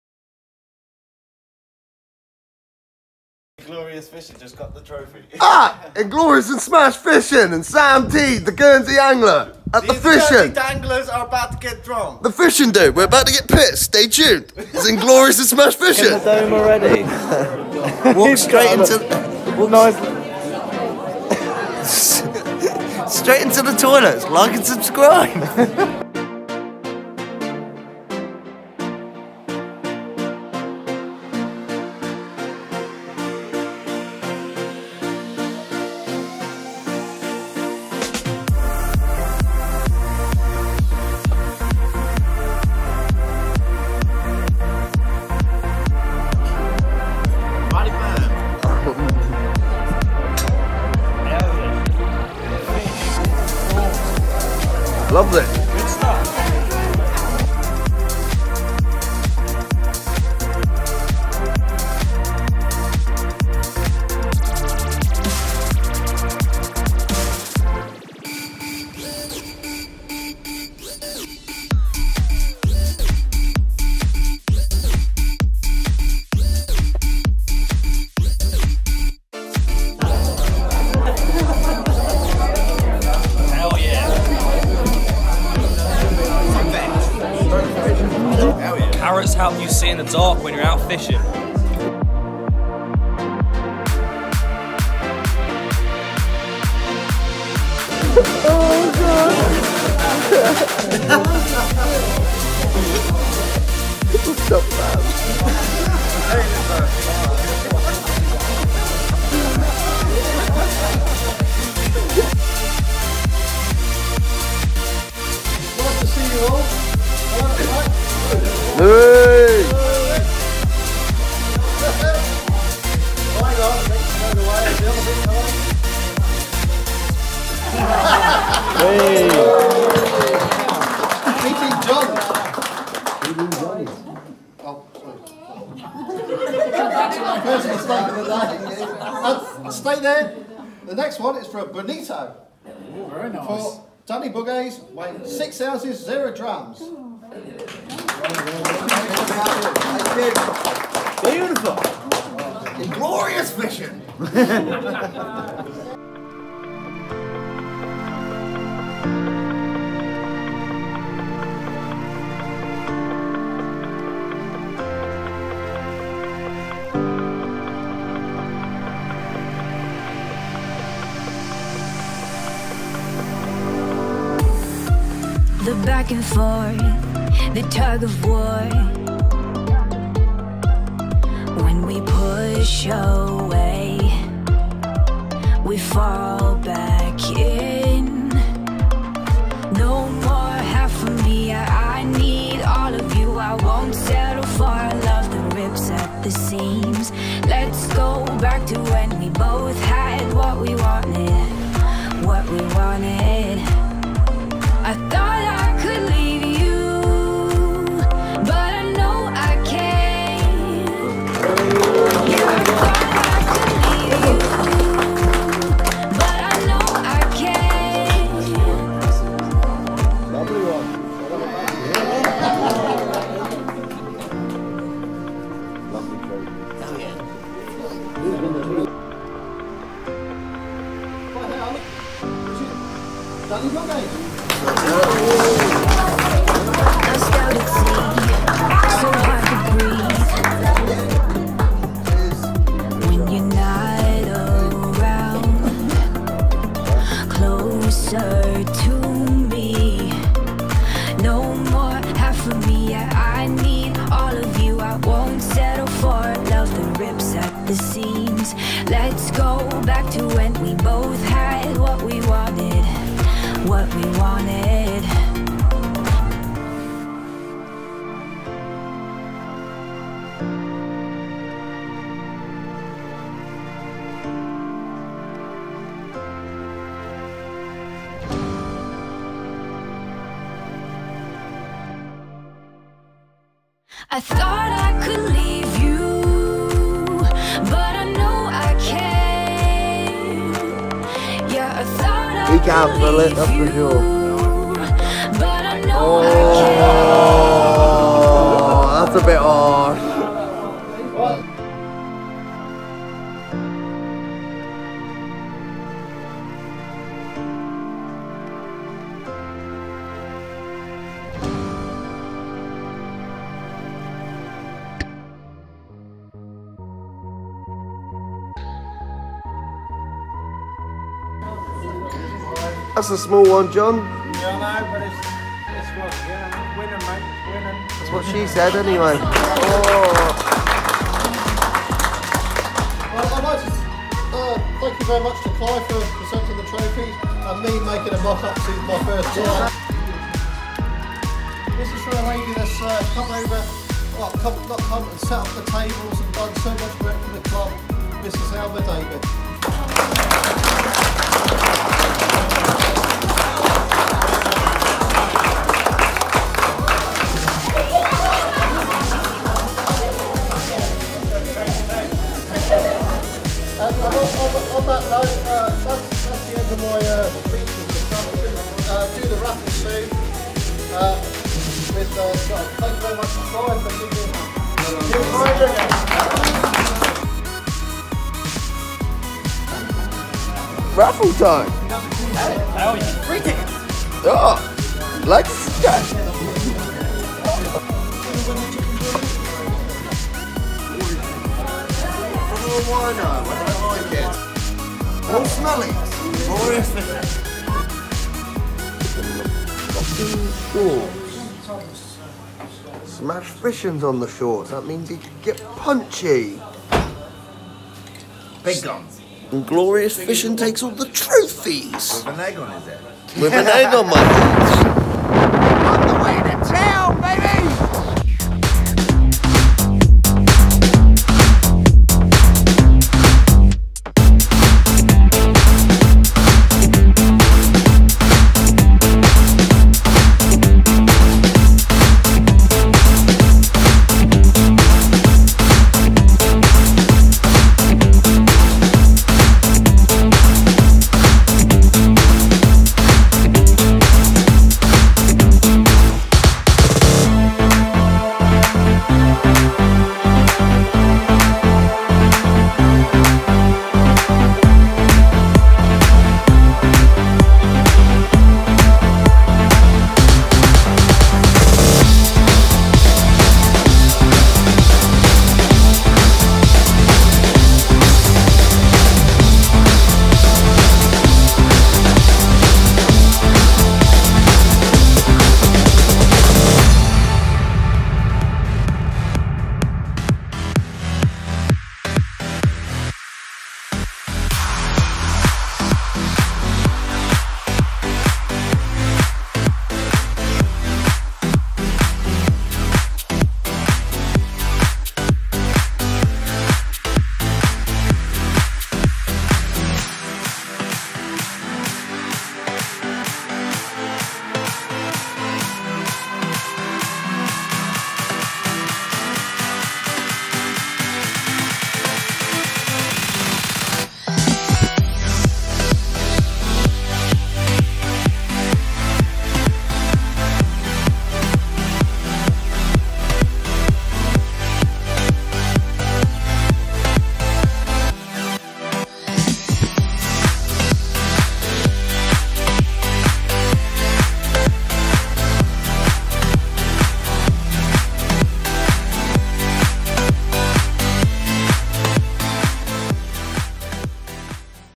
Fishing Awards Night With Smash Fishing!
Hey guys, this is just a video of last nights awards night!!! Great night with @smashfishing @theguernseyangler Jay and Sam!!! Great Night Out